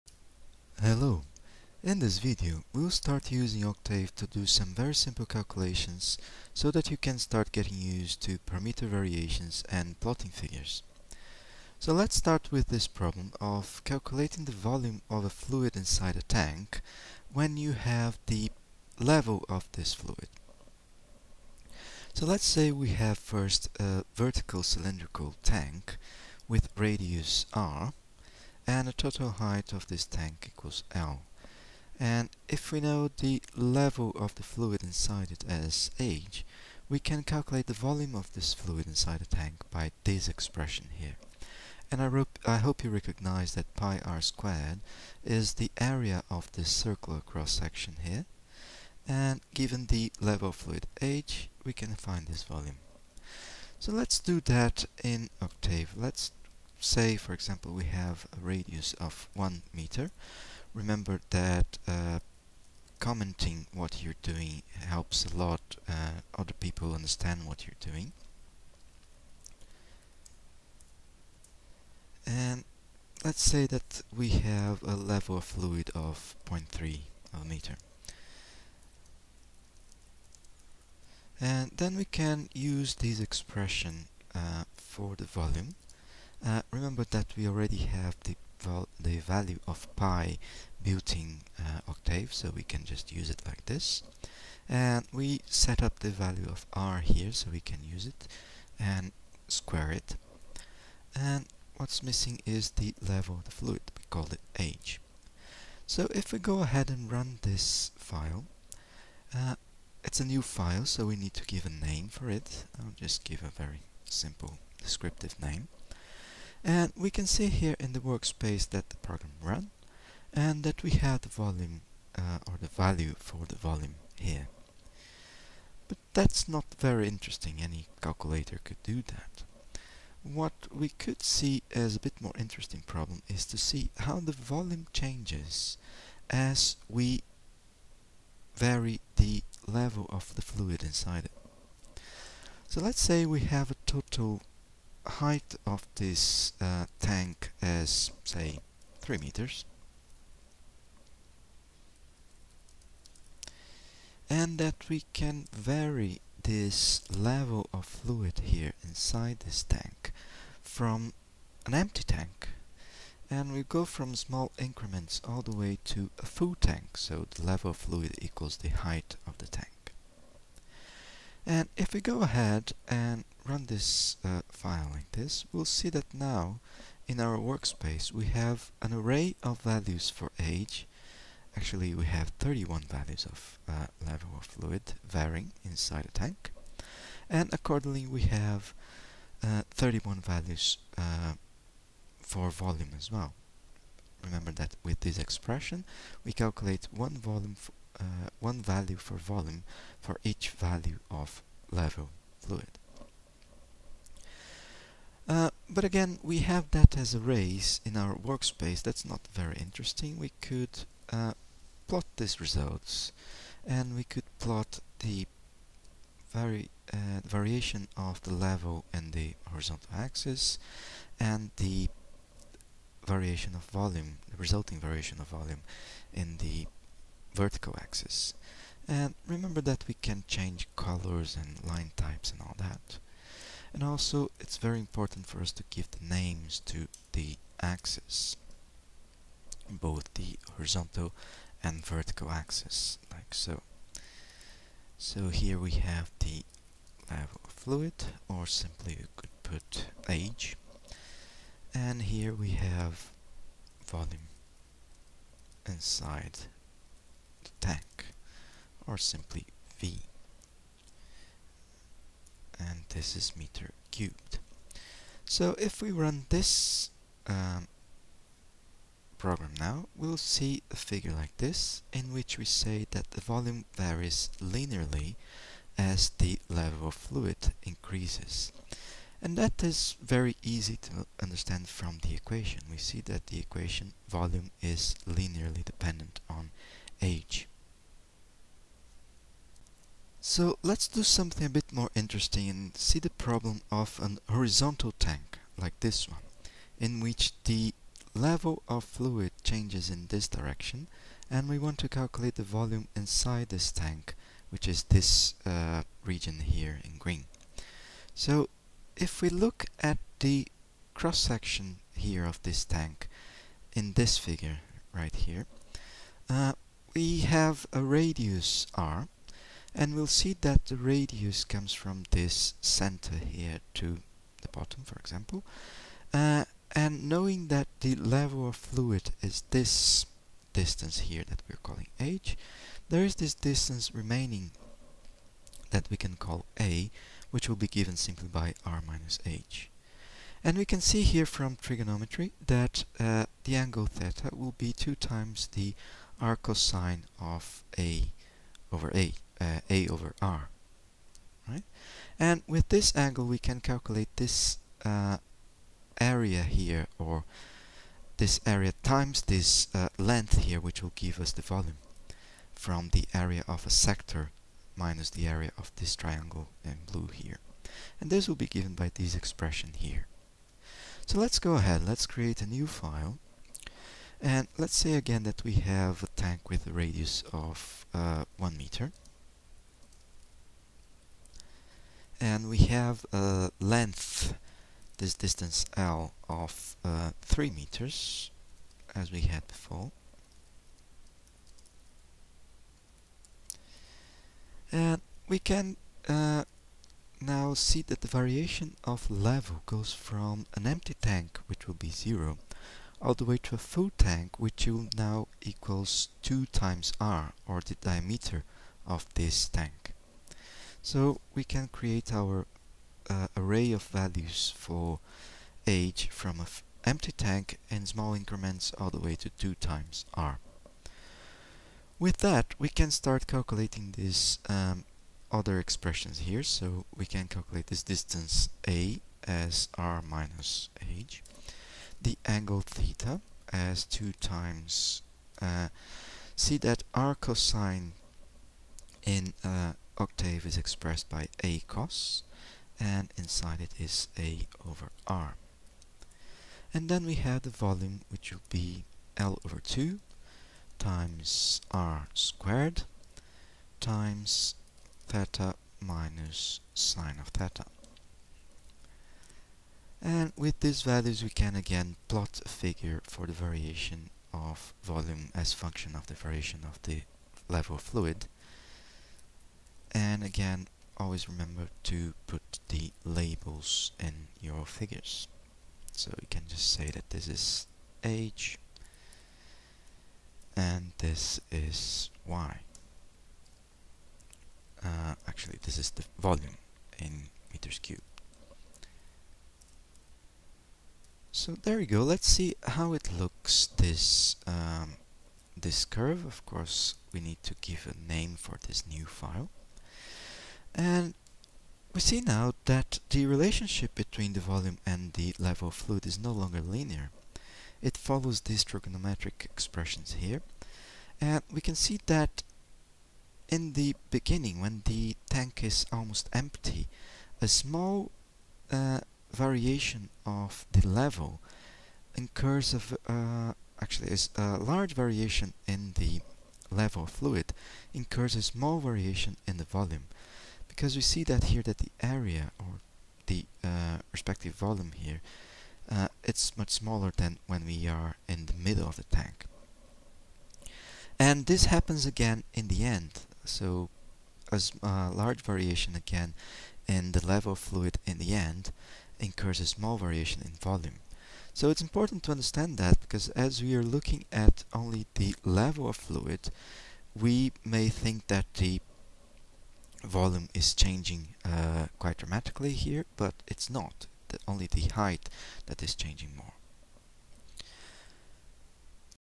octave 02 volume inside tank
octave for engineering computations - calculating the volume of fluid inside vertical and horizontal tanks.